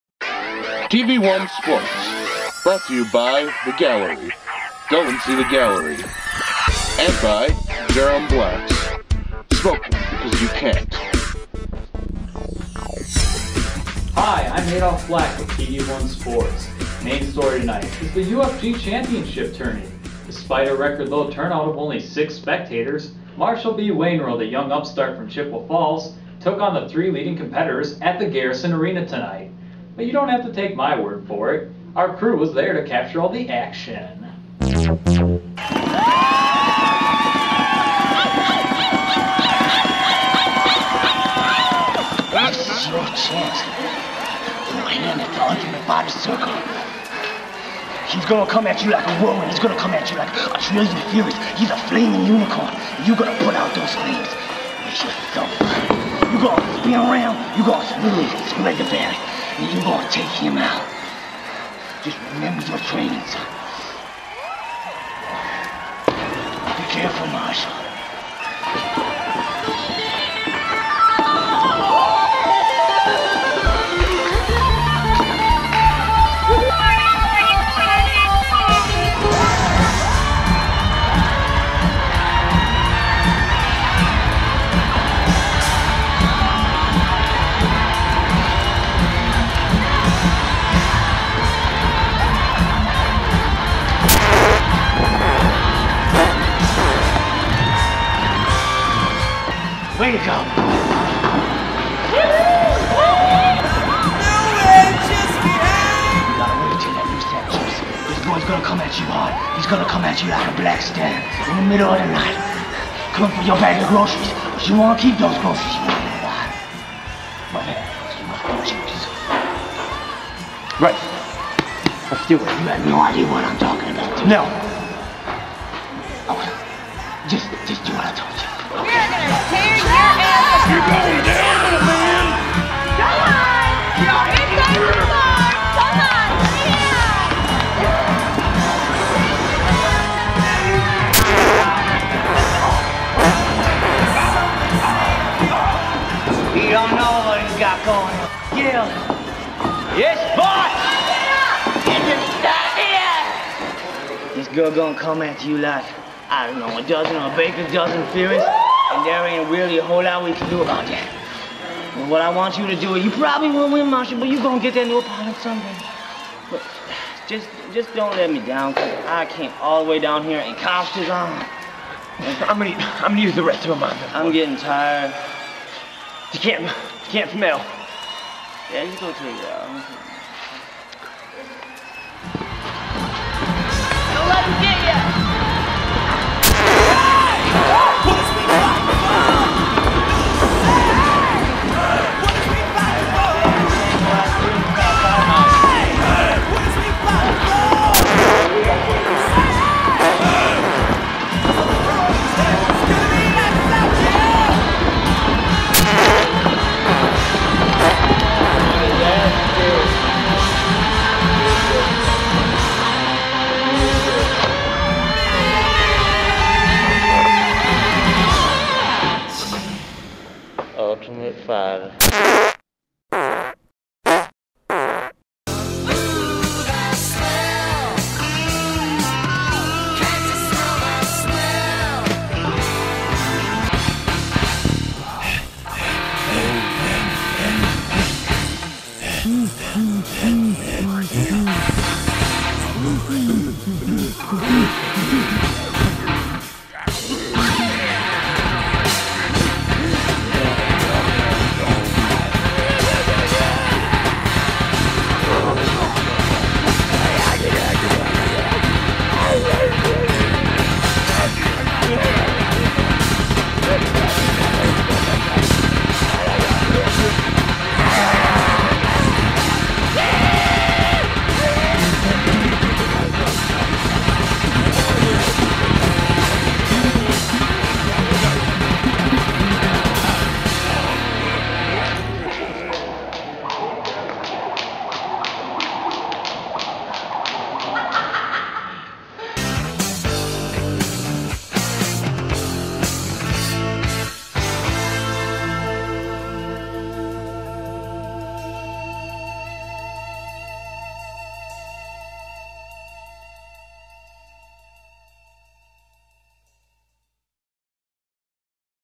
Marshall Farts part 2
Short film about dude who knocks out his opponents with farts. (Mostly) shot and edited in 48 hours on October 2-4. PG-13, 13 minutes.